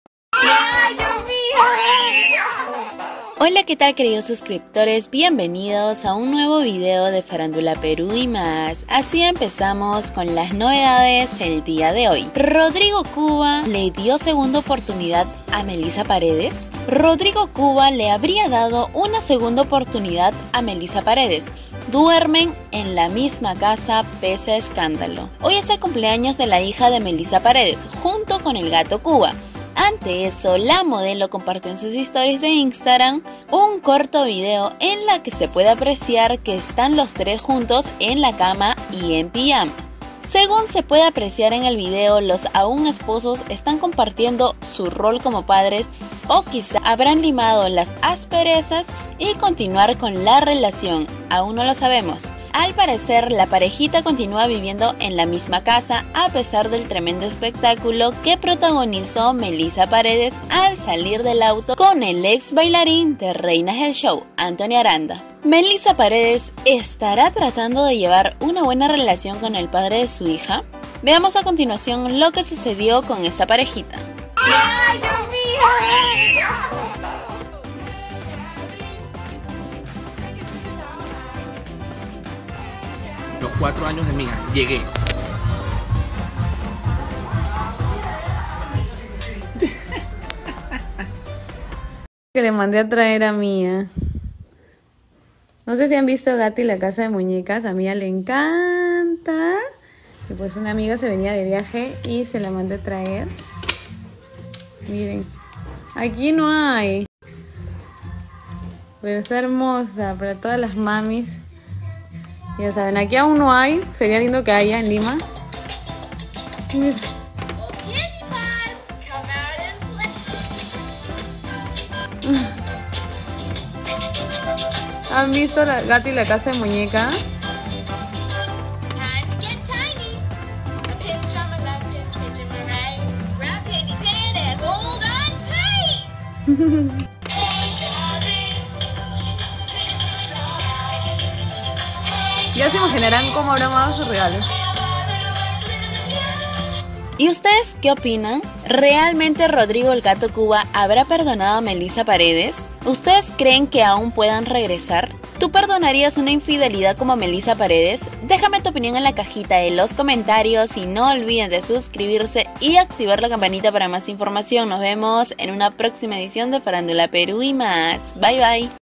EN SHOCK ! MELISSA PAREDES Y SU AUN ESPOSO RODRIGO CUBA SE MUESTRAN MAS JUNTOS QUE NUNCA
SORPRENDENTE MELISSA PAREDES Y EL RODRIGO EL GATO CUBA SE MUESTRAN MAS JUNTOS QUE NUNCA AUN DUERMEN JUNTOS YA QUE UN ESTADO DE MELISSA LO CONFIRMARIA Y PASARON DE LO MEJOR EN EL CUMPLEAÑOS NUMERO 4 DE SU HIJA .